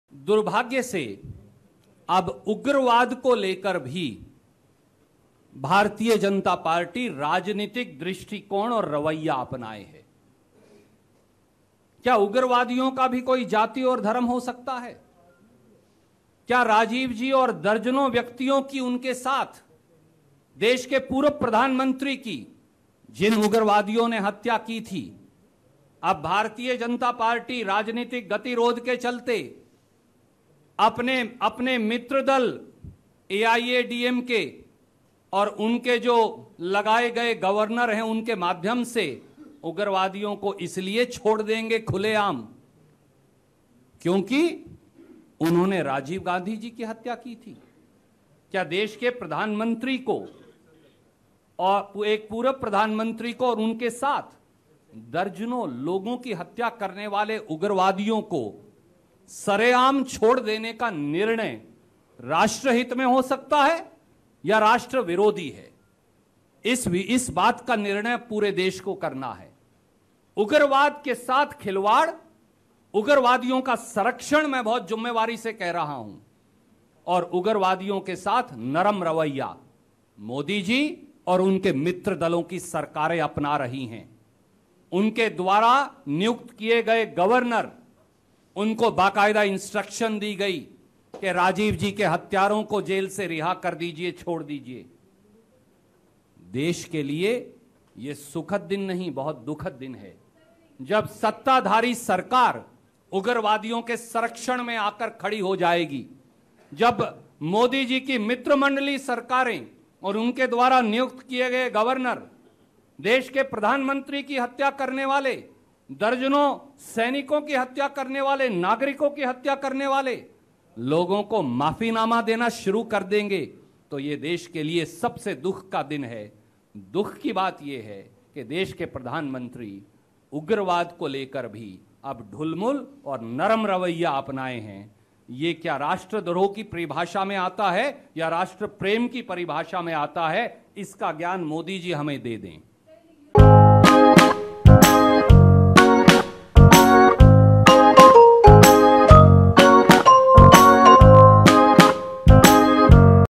Modi helps to release all 7 convicts in Rajiv Gandhi case
Will BJP now with help of friend AIADMK&their appointed governor release terrorists just because they murdered former PM Rajiv Gandhi&others along with him?: Randeep Surjewala, Congress, on TN cabinet recommendation for release of all 7 convicts in Rajiv Gandhi assassination case